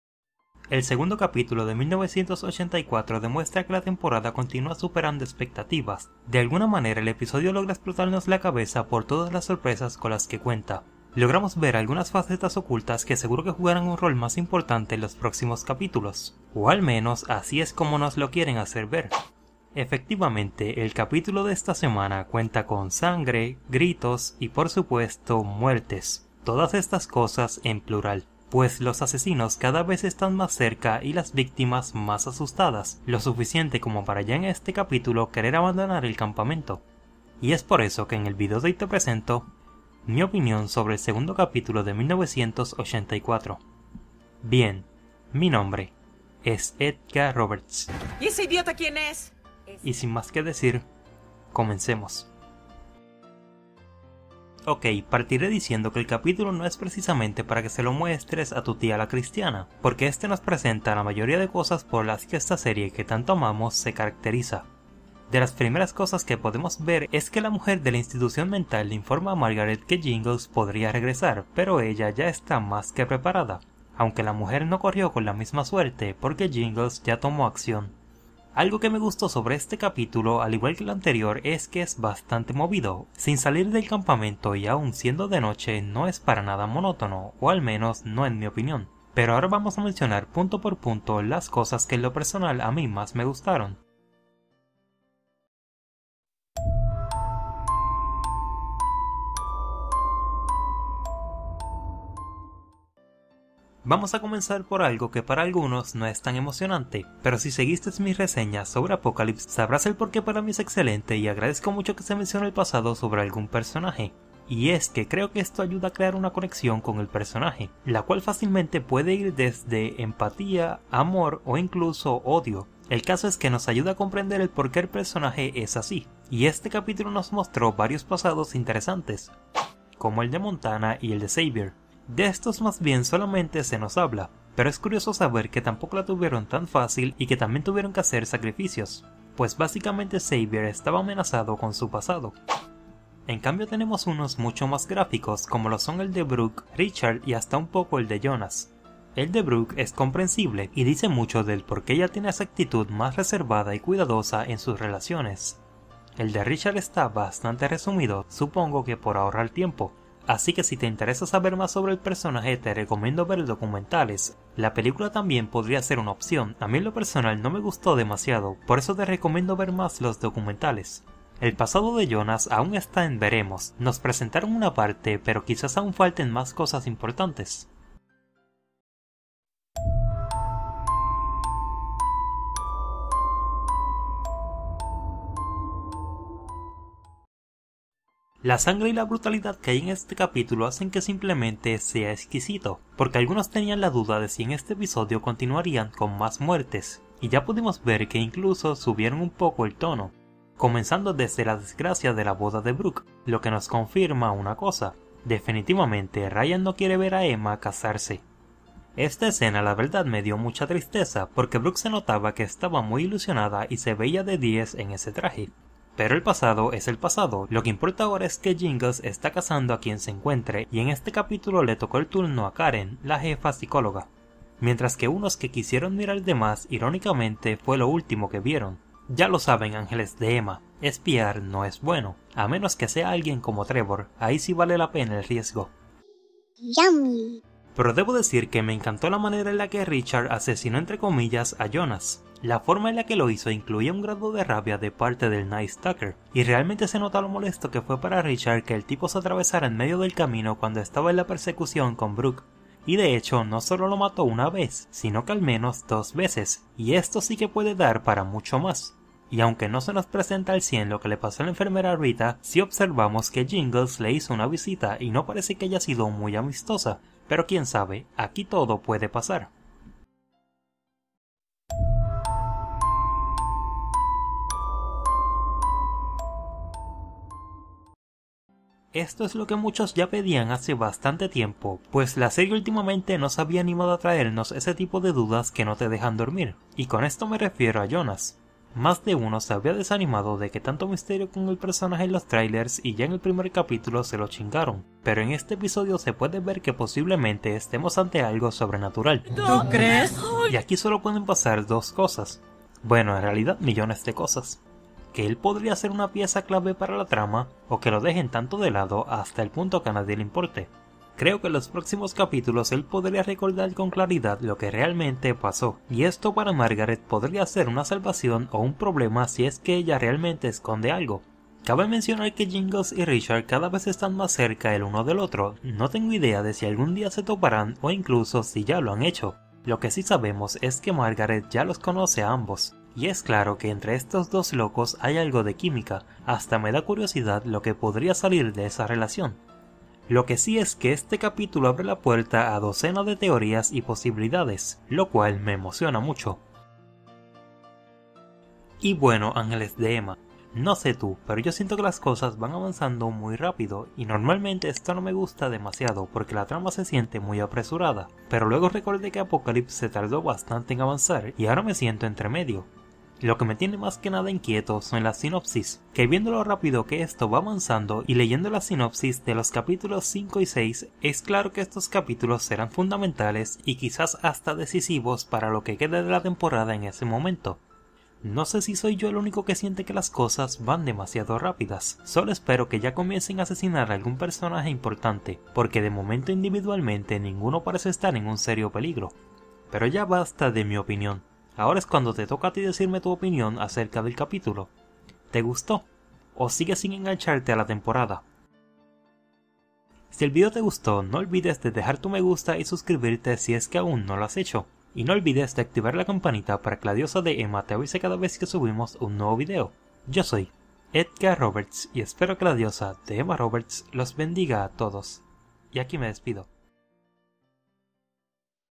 AHS 1984: Comienza El Terror En El Campamento Redwood (9X02) (Reseña con Spoiler)
Esta semana American Horror Story nos trajo el segundo capitulo de su recién estrenada novena temporada, y este continúa creando más espectativas y más dudas sobre lo que pasará en sus futuros episodios. pues tenemos personjes bastante llamativos y todos van tras de algo... Unos de su supervivencia, y otros tras venganza... El punto es que las cosas para esta temporada pintan muy bien y hasta el momento podemos sacar miles de teorias sobre la trama y sobre los secretos que ocultan los personajes

Aquí puedes comprar las Temporadas de AHS desde la 1 hasta la 7

Contacta conmigo

Agregame en AHS Amino
EdgarRoberts 98